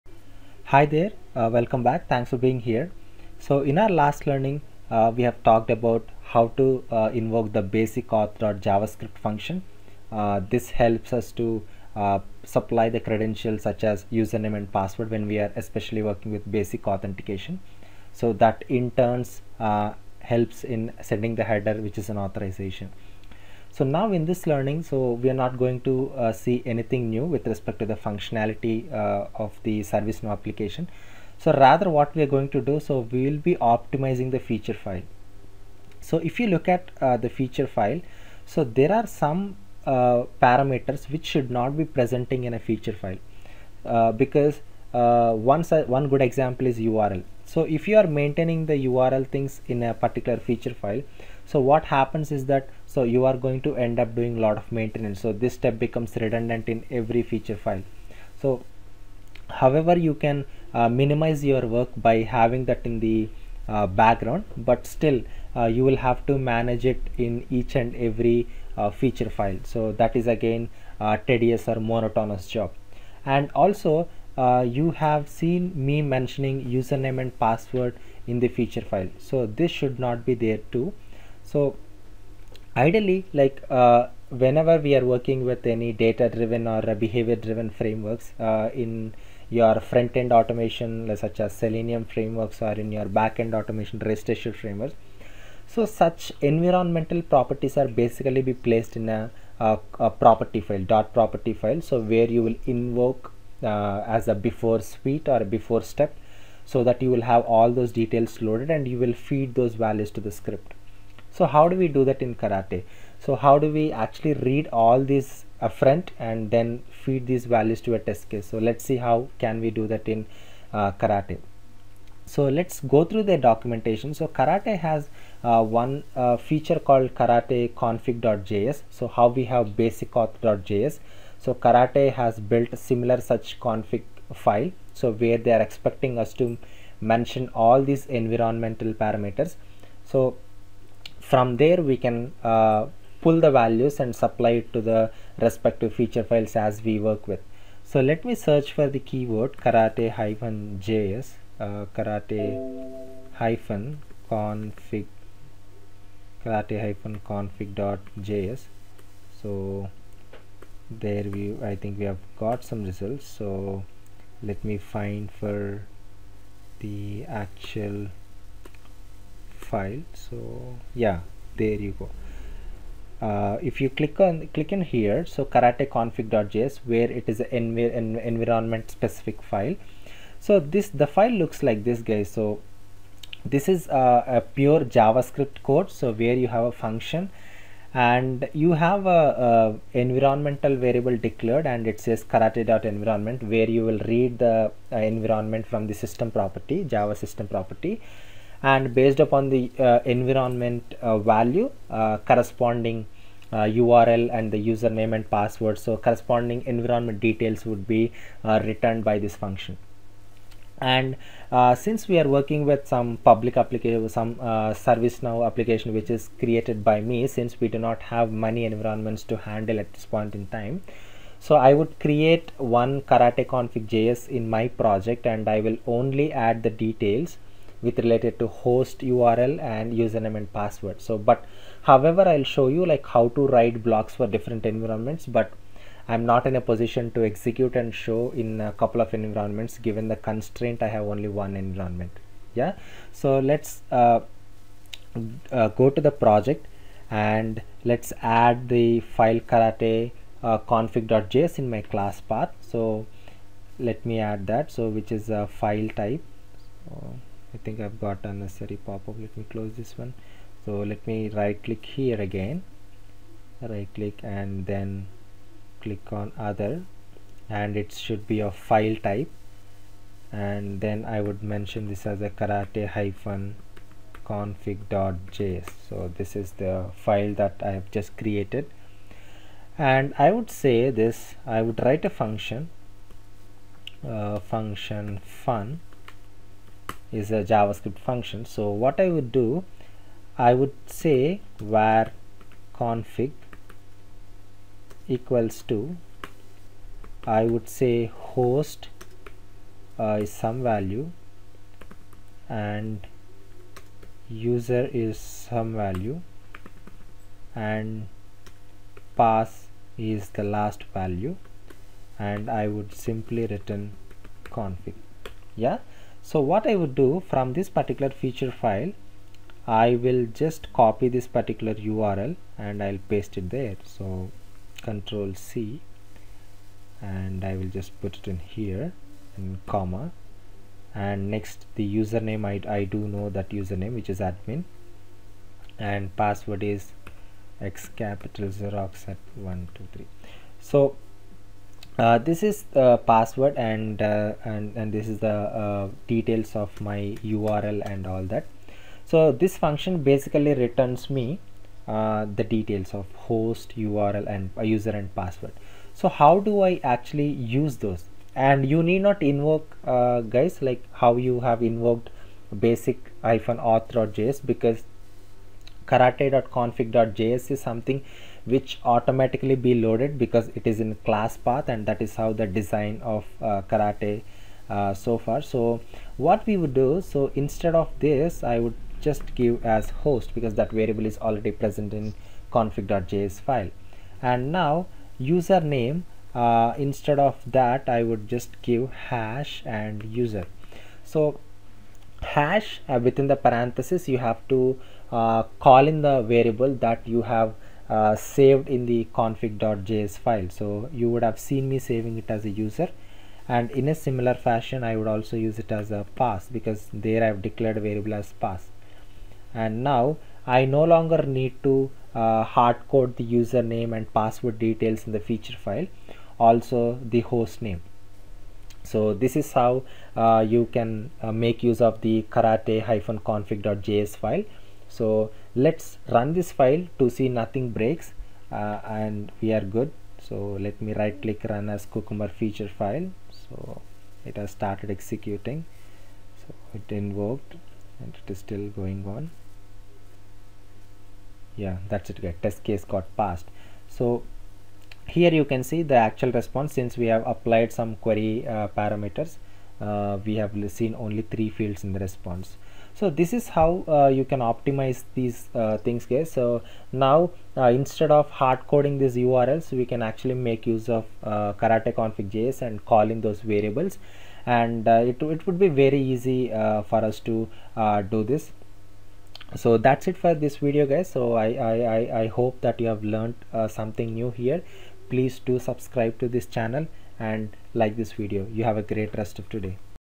How to specify environment variables and call karate-conig file in Karate test automation framework
Explained how to add the karate-config.js file to the class path
Explained how to write a config block and specify url, credentials in the karate config file.